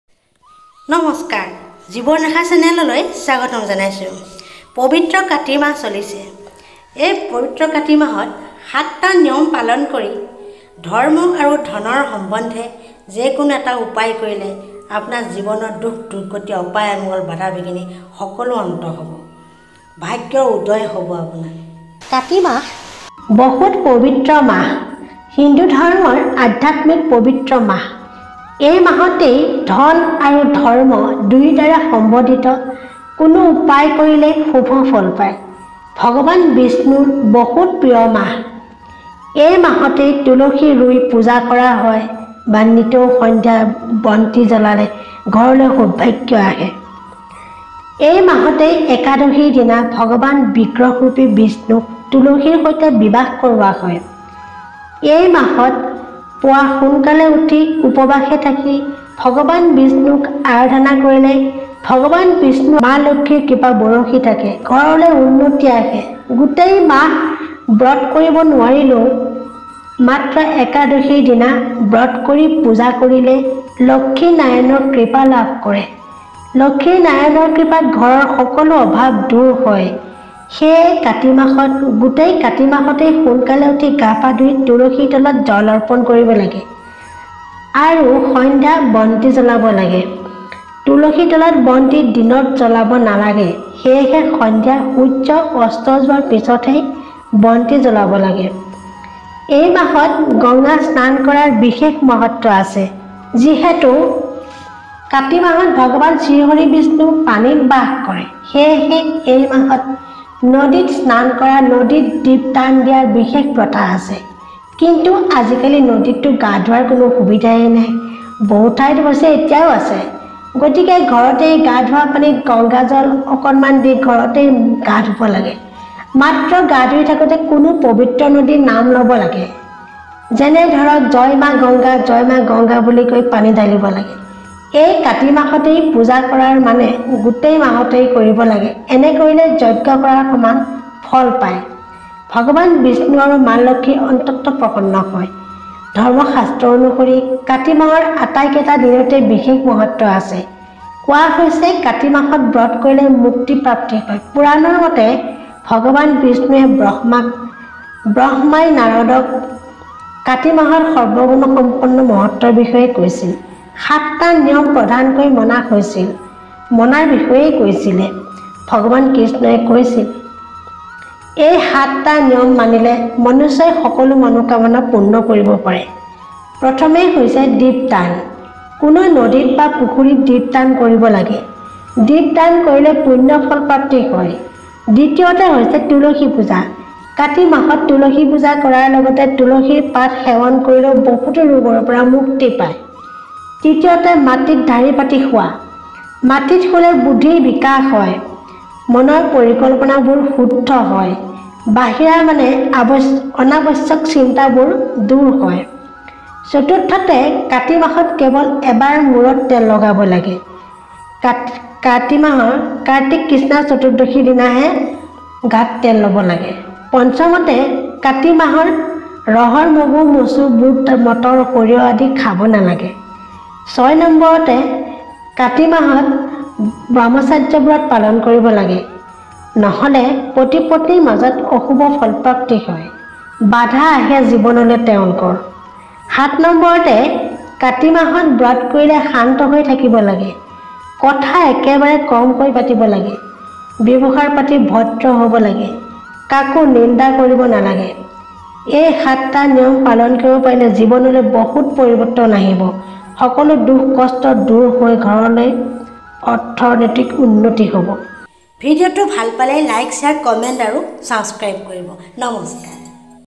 ভগৱান শ্ৰীকৃষ্ণই কৈছিল, পবিত্ৰ কাতি মাহত এই সাতটা নিয়ম পালন কৰিলে লক্ষ্মী নাৰায়ণৰ কৃপা সদায়েই থাকিব।
ভগৱান শ্ৰীকৃষ্ণই কৈছিল, পবিত্ৰ কাতি মাহত এই সাতটা নিয়ম পালন কৰি, ধন আৰু ধৰ্মৰ সম্বন্ধিত কোনো উপায় কৰিলে লক্ষ্মী নাৰায়ণৰ কৃপা সদায়েই থাকিব।